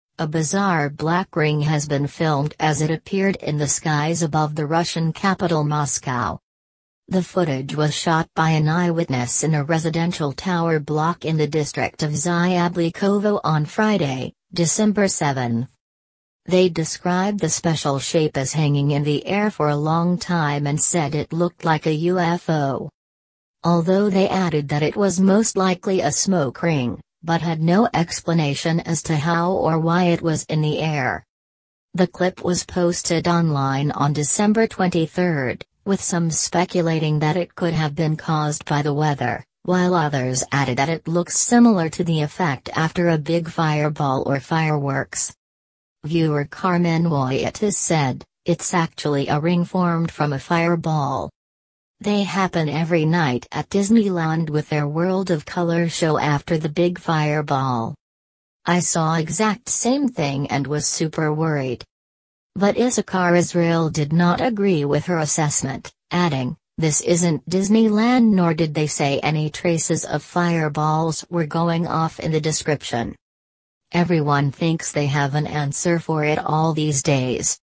Strange black ring spotted in skies above Russia but is it a UFO?
A bizarre black ring has been filmed as it appeared in the skies above the Russian capital Moscow.
The footage was shot by an eyewitness in a residential tower block in the district of Zyablikovo on Friday, December 7.
They described the "special shape" as hanging in the air for a long time and said it "looked like a UFO".